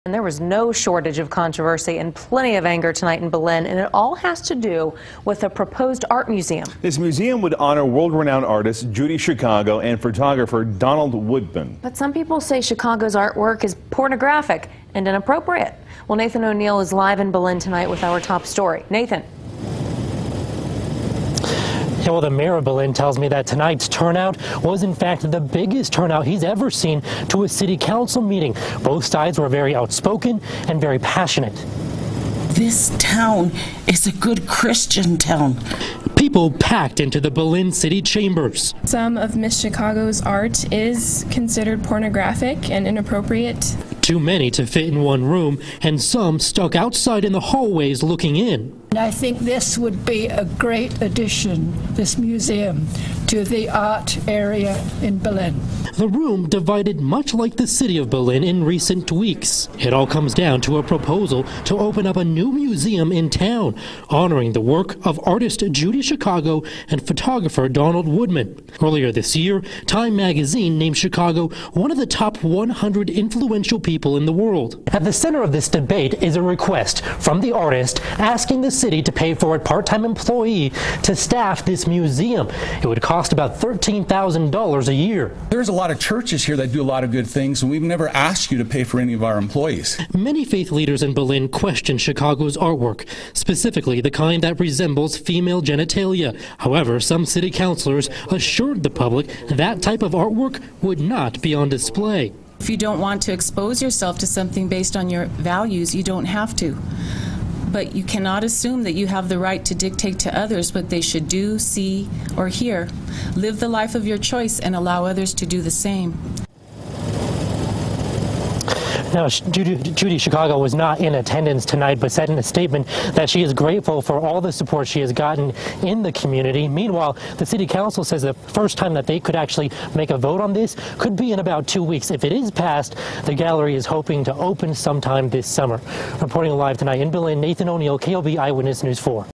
Controversial artist sparks debate about potential art museum in Belen
BELEN, N.M. - People packed into the Belen City Chambers to voice their opinion on a proposed art museum in honor of two internationally acclaimed artists.

The museum would honor world-renown artist Judy Chicago and photographer Donald Woodman. However, some say Chicago’s artwork is pornographic and inappropriate.

Earlier this year, Time magazine named Chicago one of the Top 100 Influential People in the World.

On Monday, so many people showed up for the hot topic discussion that city officials had to open an overflow room and some attendees still had to stand in the hallways.

The room was divided much like the City of Belen in recent week.

“This town is a good Christian town,” said a Belen resident who was wearing a green sticker that said “family values."

Another resident said, “I do believe that some of Chicago’s artwork is considered pornographic and inappropriate.”

Others carried signs in support of the art museum.

“I think that this museum would be a great addition to the art area in Belen,” said one woman.

At the center of this debate is a request from the artist asking the city pay for a part-time employee to staff the museum. It would cost roughly $13,000 per year.

“There’s a lot of churches here that do a lot of good things but we’ve never asked [the city] to pay for any of our employees,” said one faith leader.

Many faith leaders in Belen question Chicago’s artwork, specifically the kind that resembles female genitalia.

City councilor Ronnie Torres reassured the public that controversial artwork would not be on display.

The earliest the city council could vote on the issue is in early December. If passed, the proposed museum could open to the public as early as July 20, 2019.